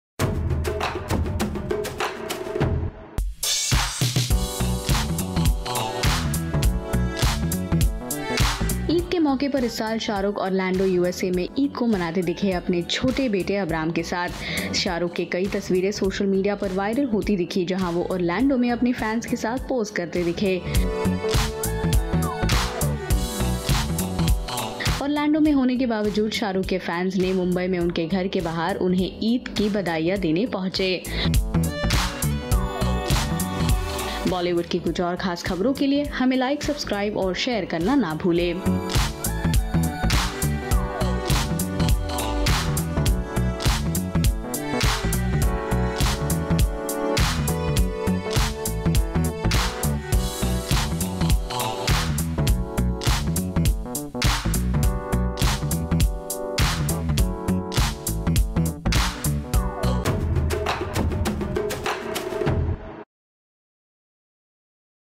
תמונות מחגיגות העיד של שארוק
שארוק חאן חגג את העיד באורלנדו.